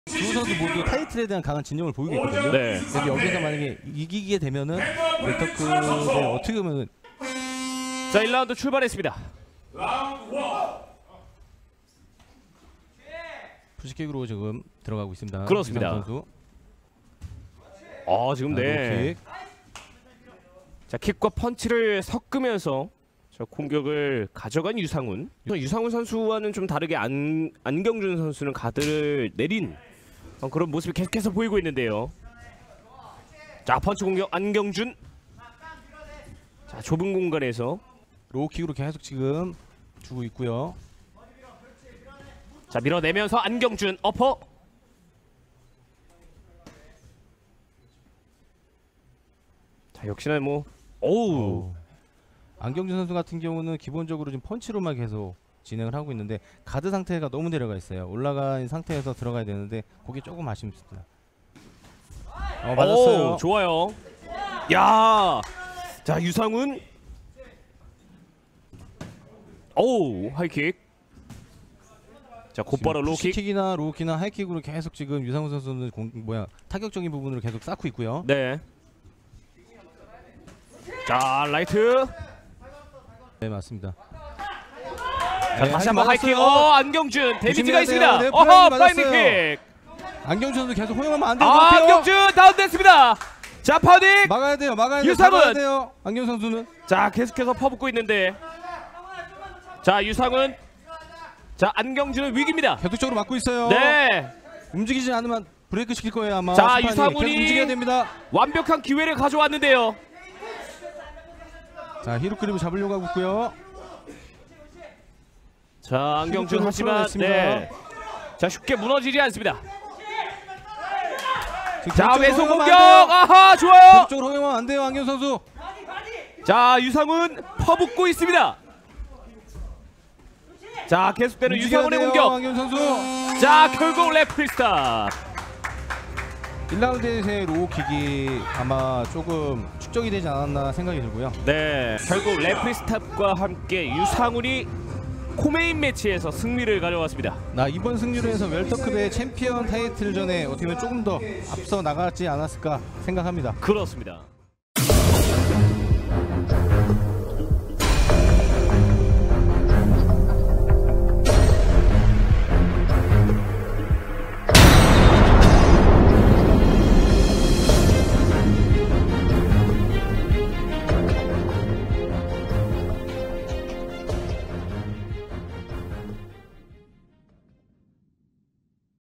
[AFC] 화려한 플라잉니킥! 유상훈의 K.O승
AFC14 안경준 vs 유상훈 경기 하이라이트